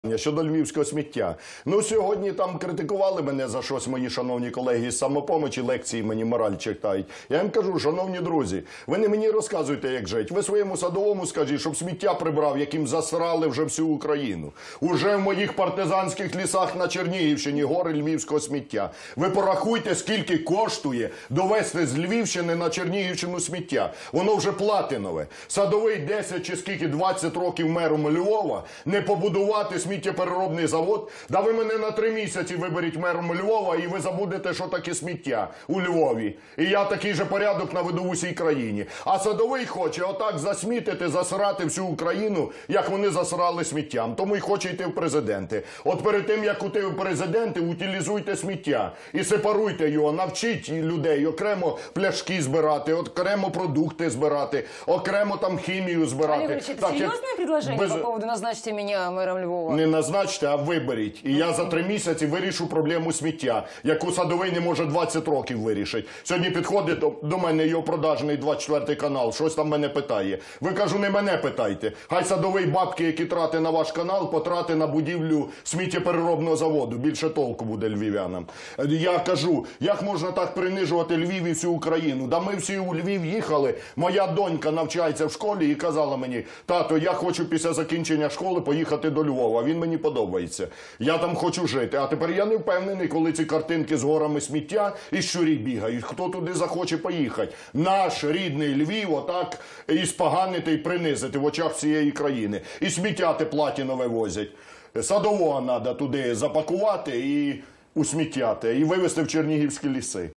Ляшко: Оберіть мене мером Львова і ви забудете про сміття!
Лідер фракції Радикальної партії Олег Ляшко в ефірі каналу "112-Україна" заявив, що вирішив би проблему зі сміттям у Львові за три місяці, якби його обрали мером цього міста.

"Тут мене критикували колеги з "Самопомочі" за щось. А я їм кажу - ви не мені розказуйте, як жить, а своєму Садовому, який засрав своїм сміттям всю Україну. Вже у партизанських лісах моєї Чернігівщини гори львівського сміття! ", - заявив народний депутат.
Ляшко наголосив, якби він був мером Львова, вирішив би питання зі сміттям за три місяці.
"Садовий за 10 років на посаді мера Львова не побудував сміттєпереробний завод. Та ви мене на три місяці оберіть мером Львова і ви забудете, що таке сміття! Я такий же порядок наведу і в усій Україні. А Садовий хоче так засмітити і засрати Україну, як він засрав Львів", - обурився Ляшко.
Політик також розповів, що його донька, яка ще навчається у школі, мріяла після закінчення навчання жити у Львові. І йому прикро, що тепер улюблене місто його дитини перетворили на смітник.